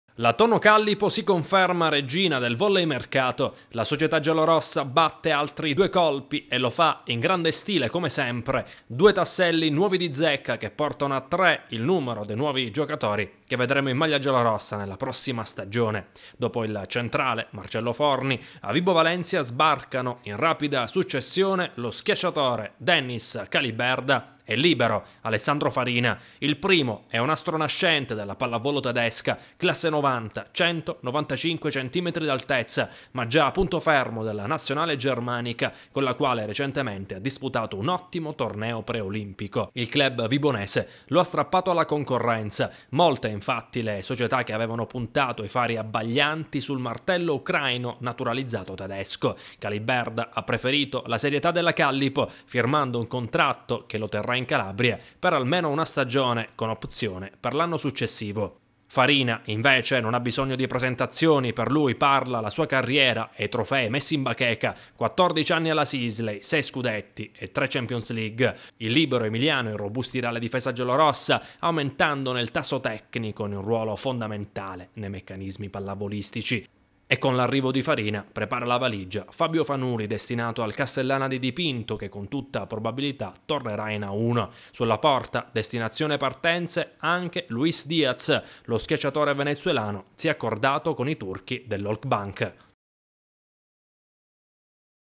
"TONNO IN...FARINA...TO ALLA BIRRA (TEDESCA)" RK SPORT
Dopo le conferme di Blengini, Coscione e Klapwijk, l'addio di Nikolov e l'ingaggio di Forni la Tonno Callipo Vibo incastona altri due tasselli nuovi di zecca sul proprio mosaico. Si tratta dell'astro nascente della pallavolo tedesca, lo schiacciatore Kaliberda, e il libero pluricampione Alessandro Farina.